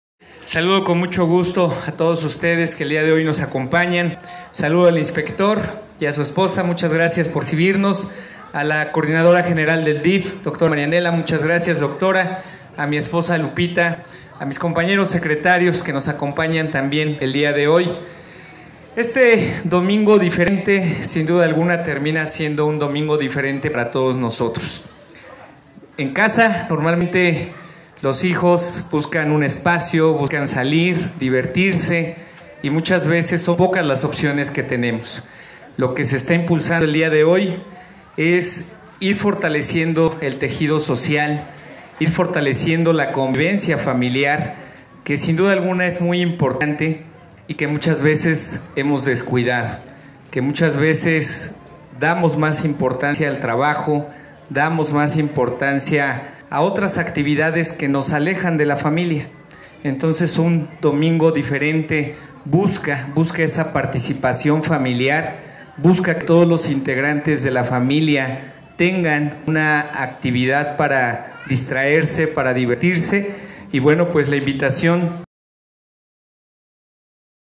EdmundoTlatehui brinda un discurso en la inauguración de "Un Domingo DIFerente"
EdmundoTlatehui, Presidente Municipal de San Andrés Cholula, brinda un discurso en la inauguración de "Un Domingo DIFerente" e invita a los cholultecas a asistir al evento